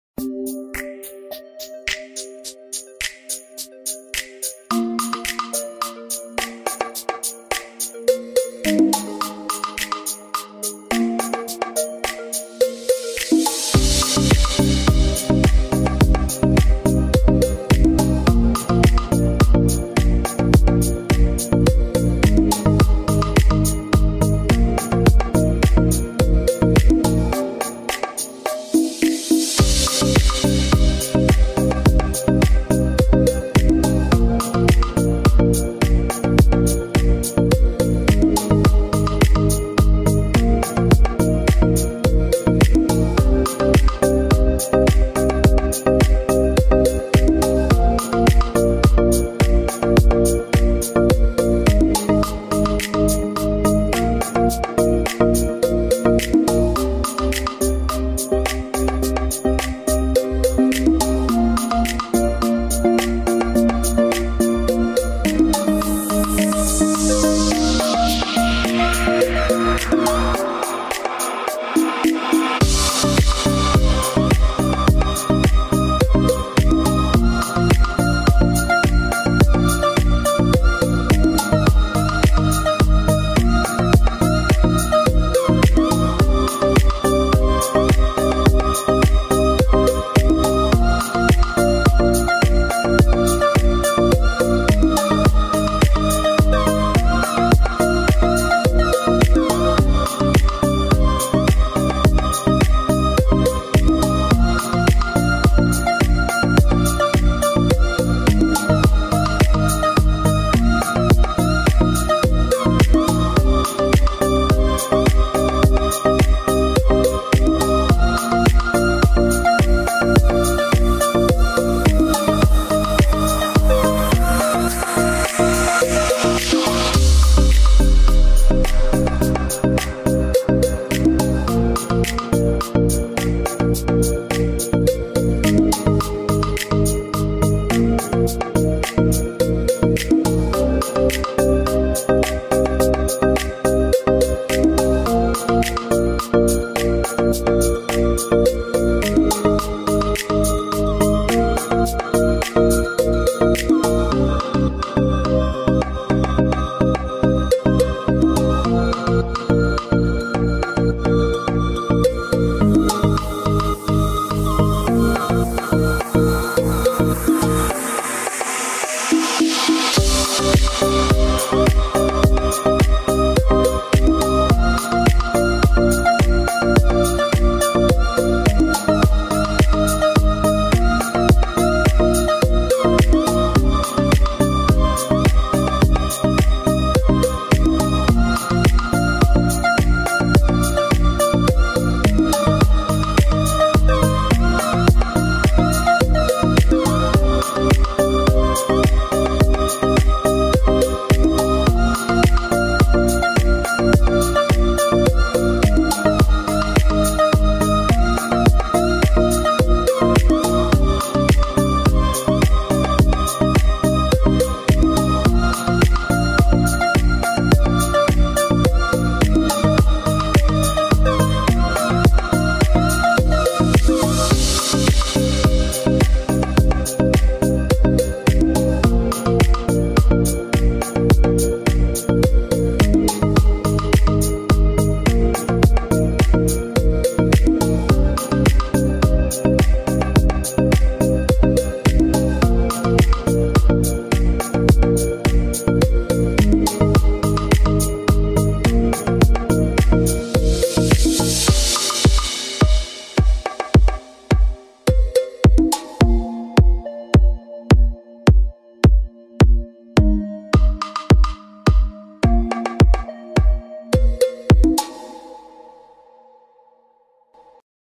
Custom Build - Haropla Green - Haro of Destiny 360 video
Completed my custom haro build! I didn't get to do everything I wanted, but it was a fun little build. Sorry I didn't film any of the process. Some real life stuff got in the way, but I am back.